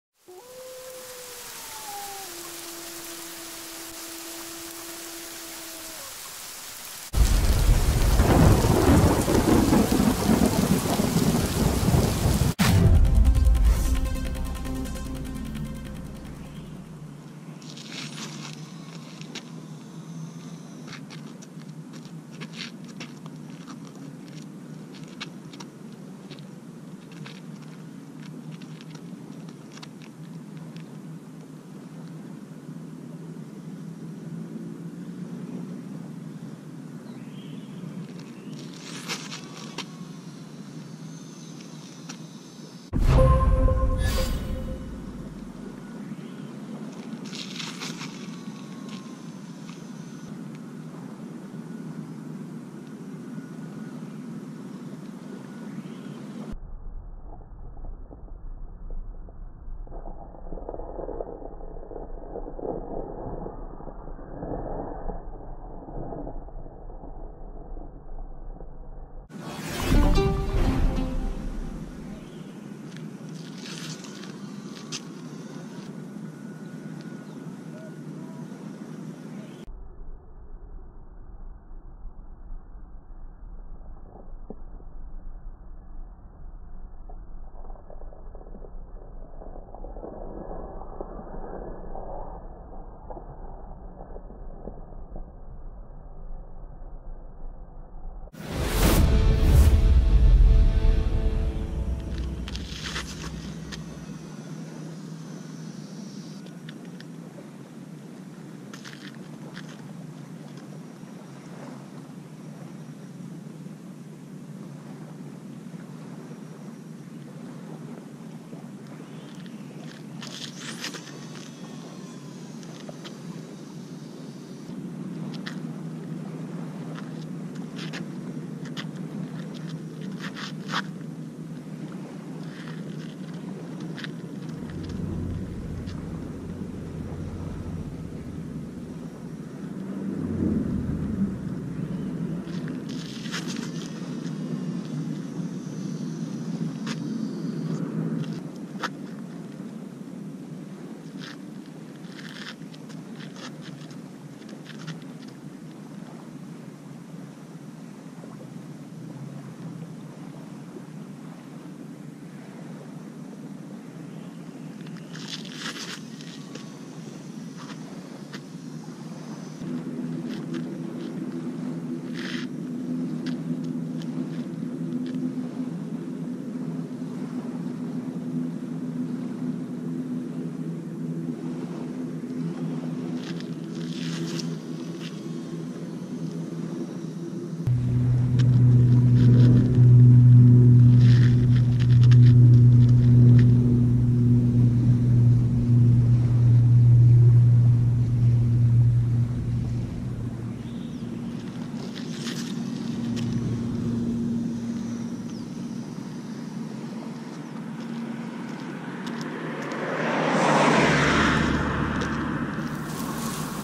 High swing Long Distance Pendulum Casting
Long Distance Pendulum Casting
Page steps up and shows us how to fire a lead out to the big blue yonder using the pendulum cast